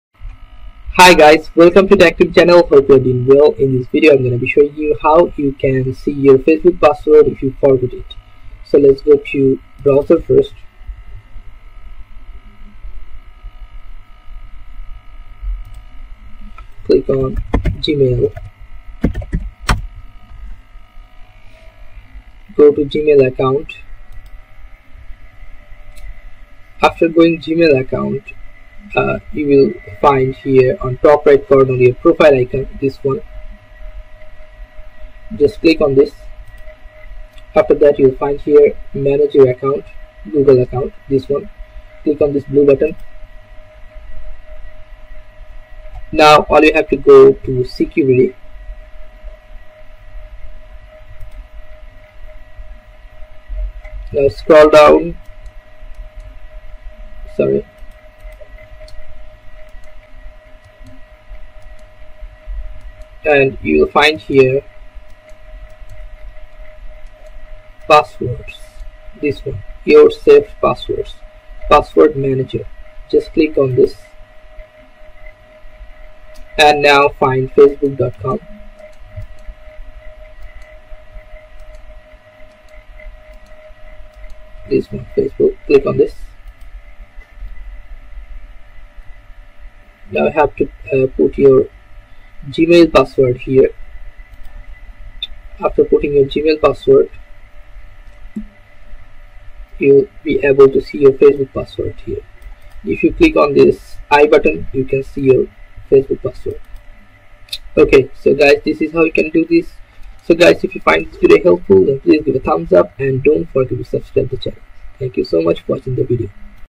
Forgot Your Facebook Password? Here's How to Recover it Safely 2025 [ New Method ]
Locked out of your Facebook account? No worries! In this video, we guide you through the step-by-step process of recovering your forgotten password securely. Discover practical tips and tools to regain access to your account without compromising your data. Don't let a forgotten password stress you out – watch now and regain control!